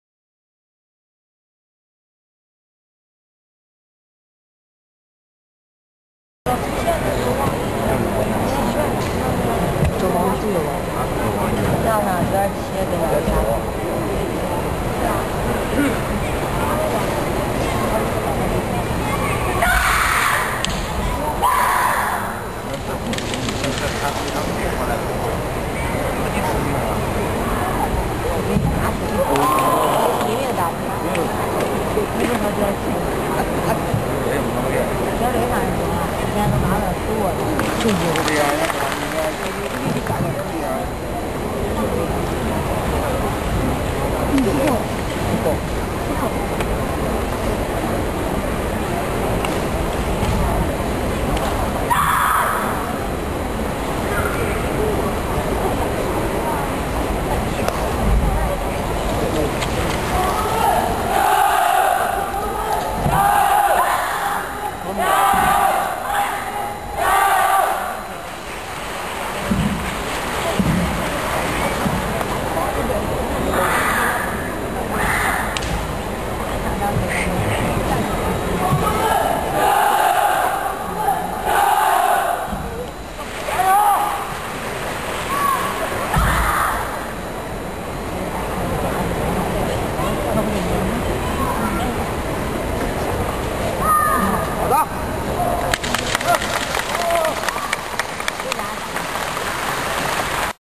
2005 All China Games: Wang Lin (Guangdong) Women's Nanquan
Wang Lin from the Guangdong Wushu Team competing in Women's Nanquan at the All China Games in Nanjing, China on October 14, 2005.  Video by: Mark Moran